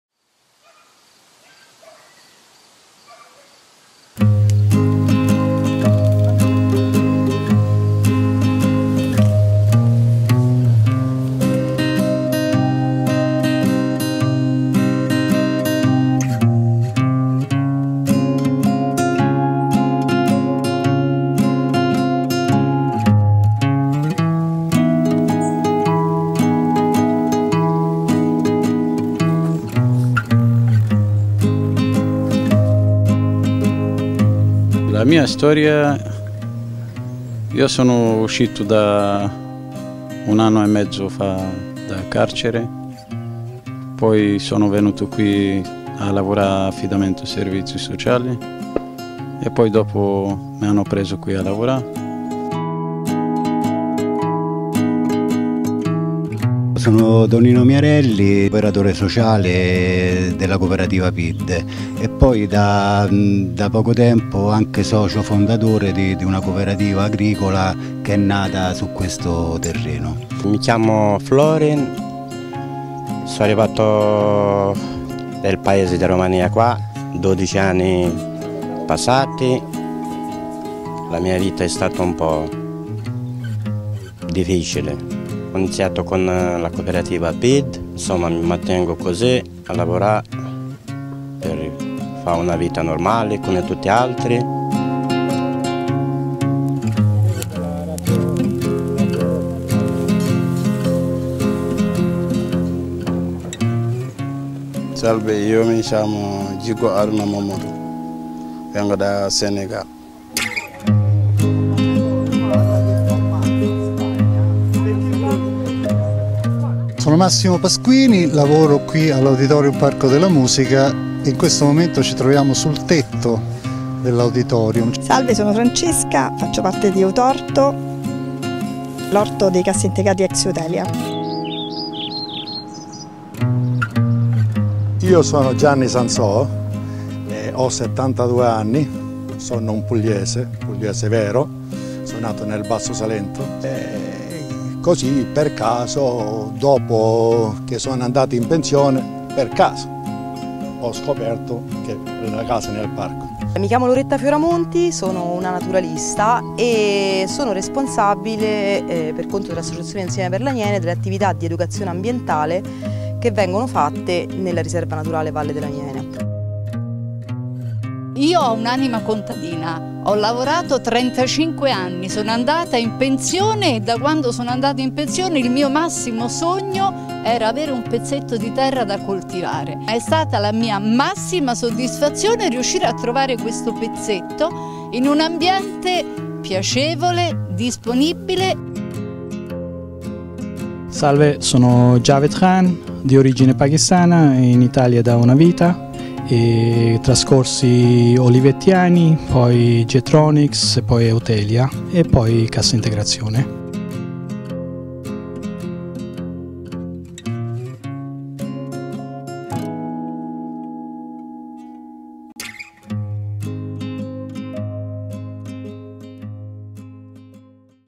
Video-interview local urban gardens (Rome)
In November 2014 a first trailer of the project was prepared for Rome. It includes a series of interviews, in already existing urban gardens/A Novembre 2014 abbiamo girato il primo trailer del progetto per Roma. Include una serie di interviste in orti urbani già esistenti. Grazie a tutti gli intervistati per la collaborazione!